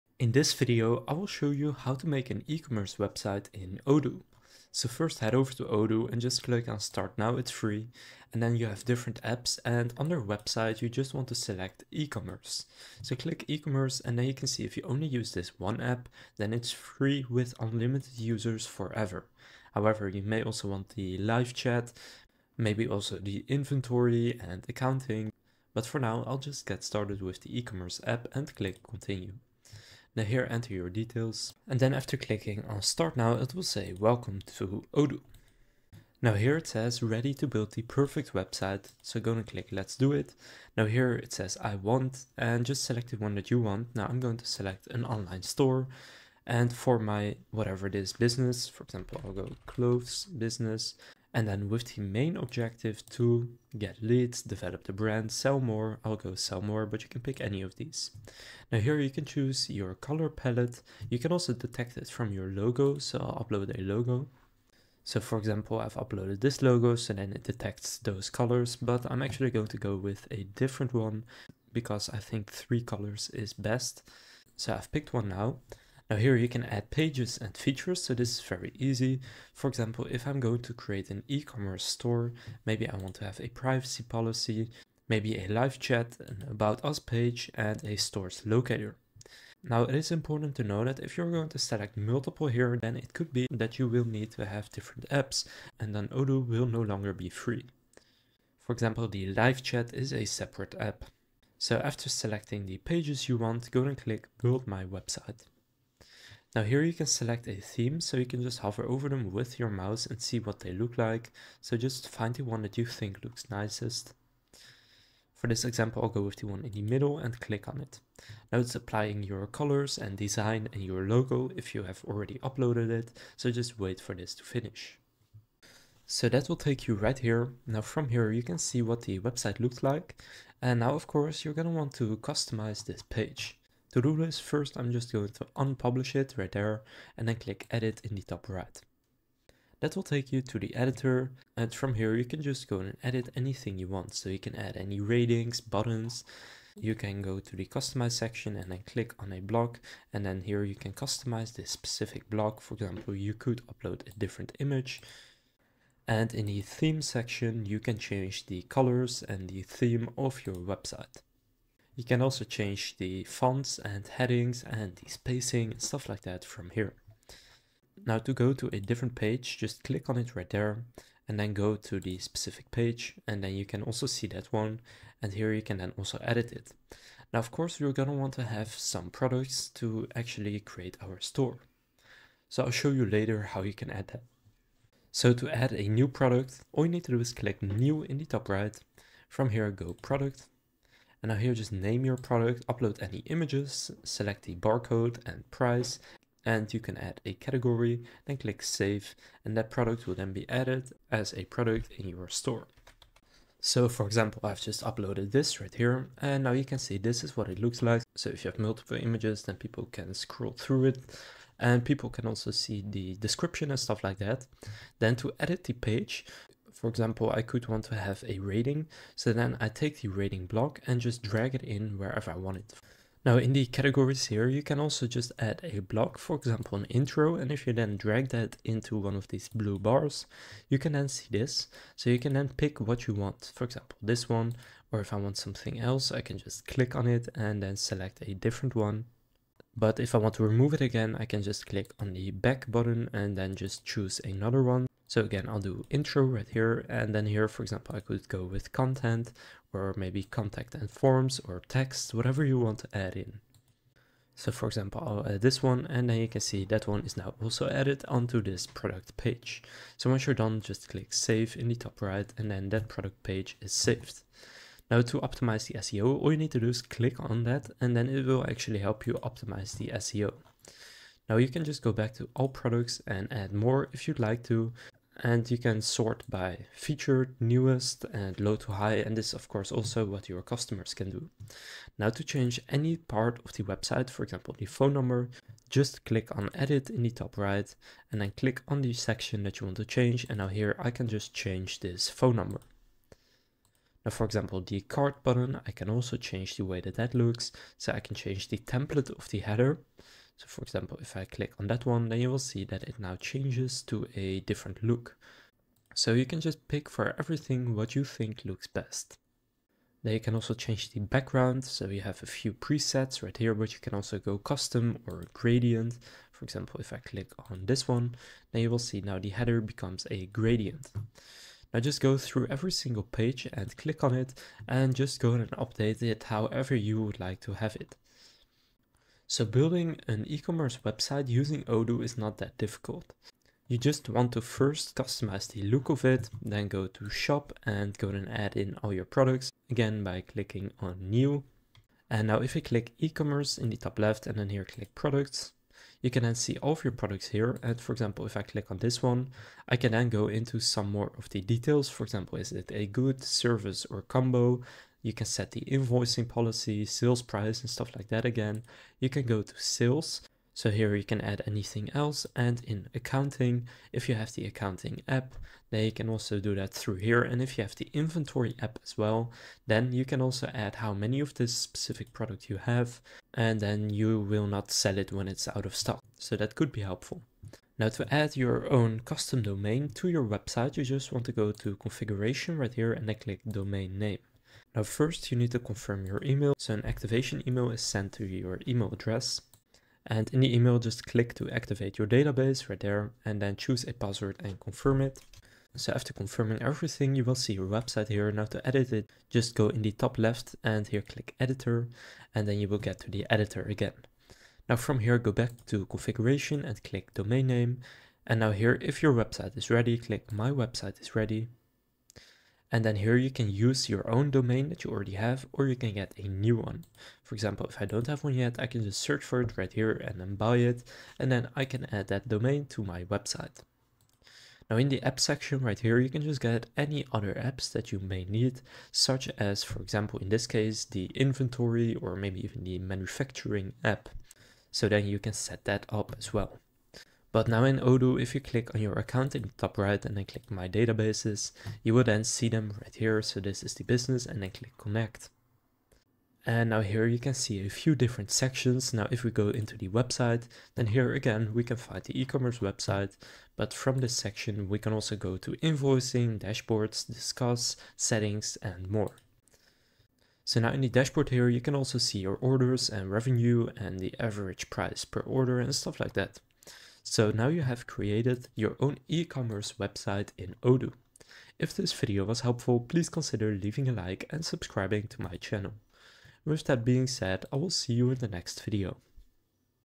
How to Make Ecommerce Website in Odoo - Step by Step
In this video I'll show you how to Make Ecommerce Website in Odoo.